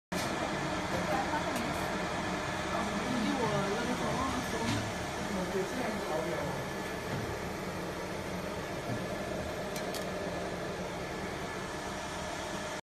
50W Rycus cutting 1mm brass
50W laser marking engraving machine, could do cut